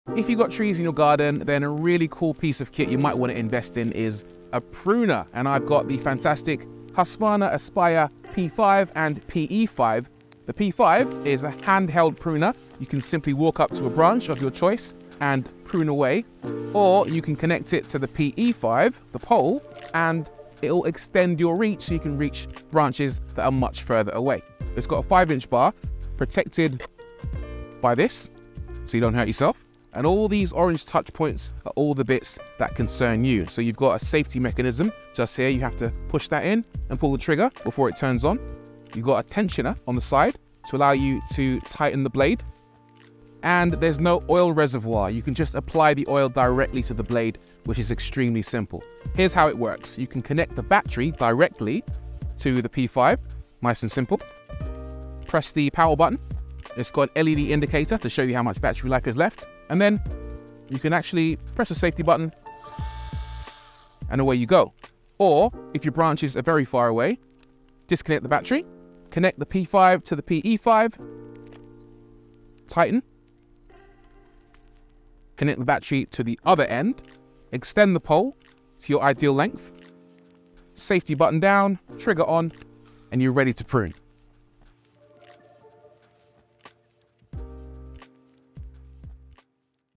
Husqvarna Aspire™ P5-P4A and PE5-P4A Battery Pruner | With Rory Reid
Motoring and technology enthusiast Rory Reid presents the new-for-2023 Husqvarna Aspire™ P5-P4A, a battery-powered compact pruner perfect for pruning trees, cutting down garden waste and smaller carpentry work. The P5-P4A can also come supplied with an adjustable telescopic pole to reach higher branches and both are compatible with the POWER FOR ALL battery system.